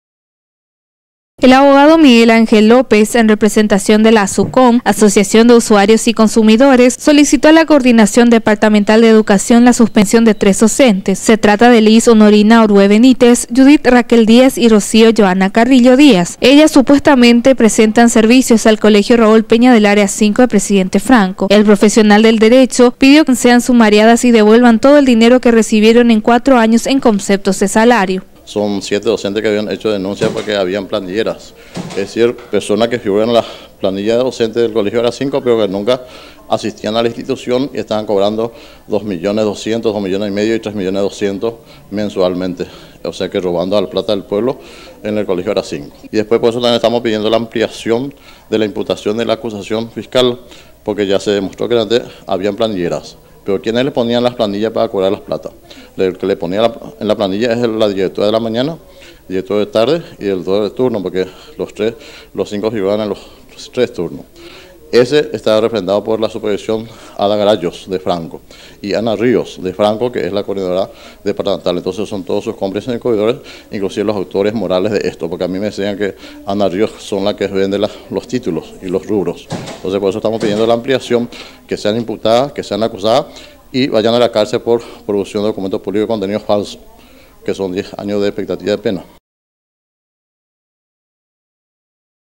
PIDEN SUSPENSIÓN DE DOCENTES PLANILLERAS
El abogado Miguel Ángel López, en representación de la Asucom, Asociación de Usuarios y Consumidores solicitó a la Coordinación Departamental de Educación la suspensión de 3 docentes.